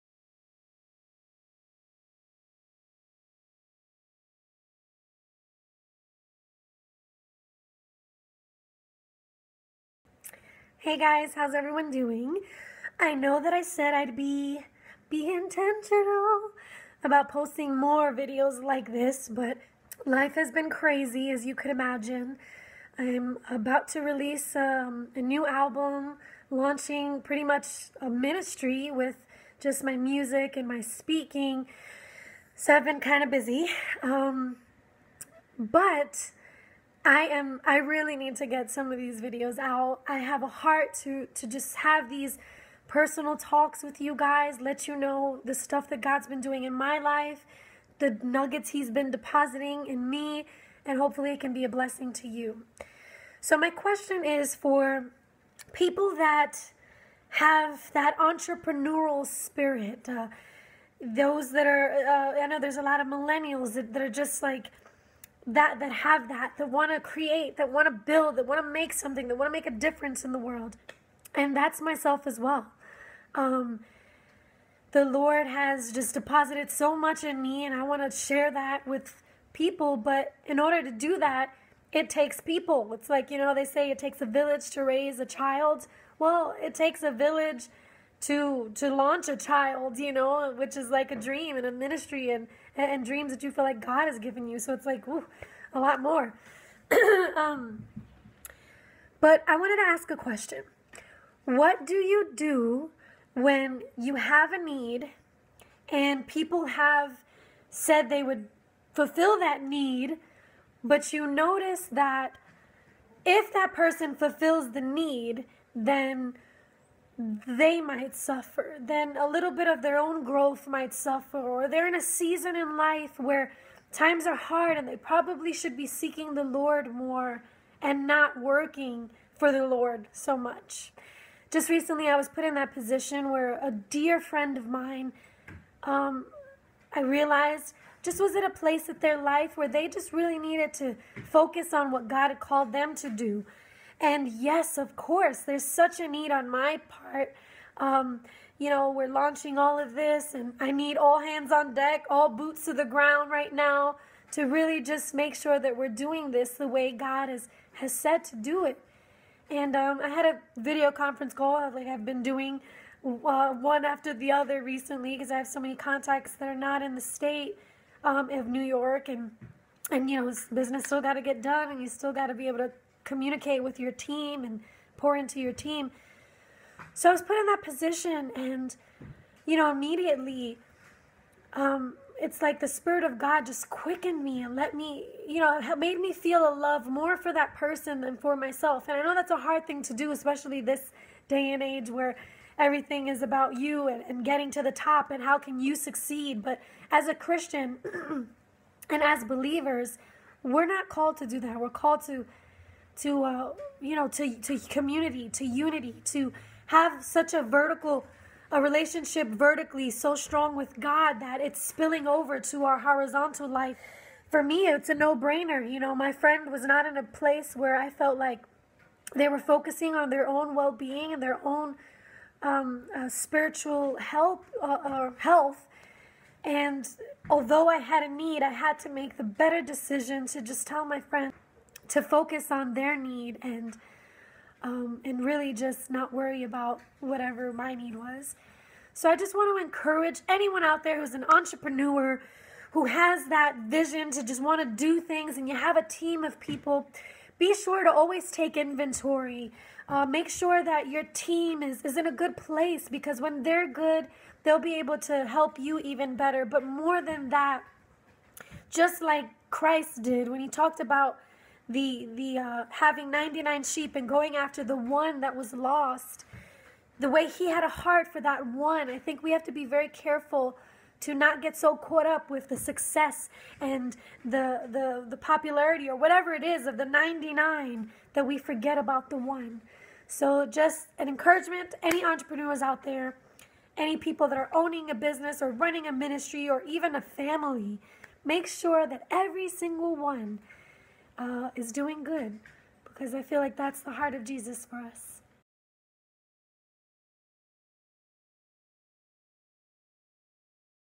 Can You Be a Selfless Entrepreneur? - Jeannie Ortega (Treasure Nuggets)
I've decided to share with my dearest friends, fans, and observers nuggets of wisdom that I've learned throughout my journey that I treasure.